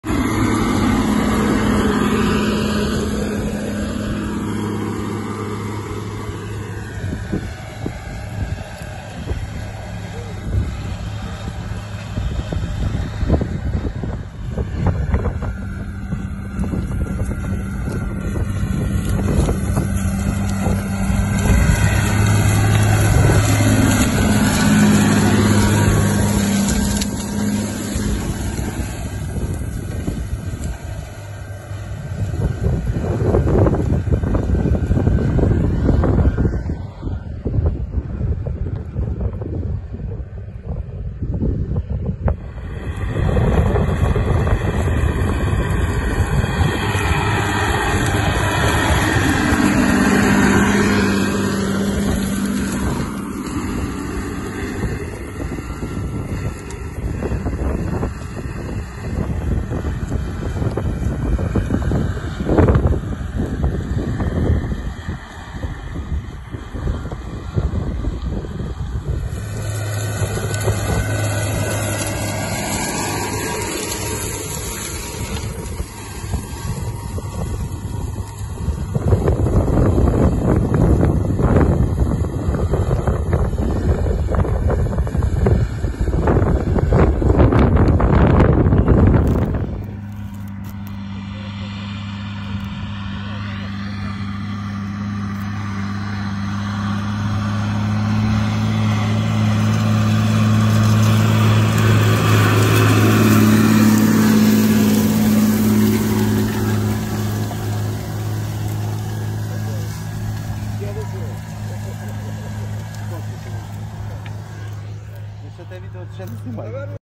Дискування стерні соняшника 2022
Дискування стерні соняшника короткобазовою дисковою бороною Poettinger Terradisc 6001T з рублячим котком Cross Cuter